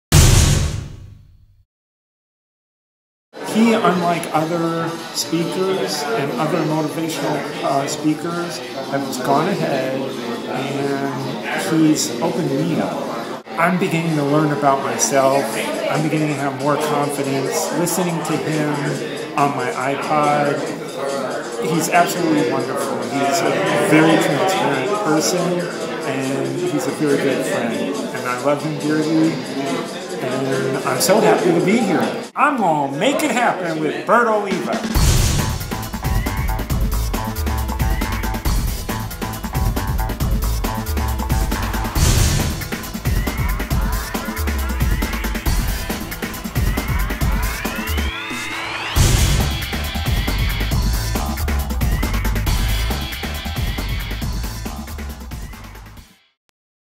BERT OLIVA: CHICAGO, IL | STEVE MILLER
Join BOWAworld in this BOWAtestimonial episode for Bert Oliva and his team. BOWAtestimonials lets people share their experiences, emotions and things learned at a Bert Oliva event!

Bert Oliva
12080 Southwest 127 Avenue
Suite 305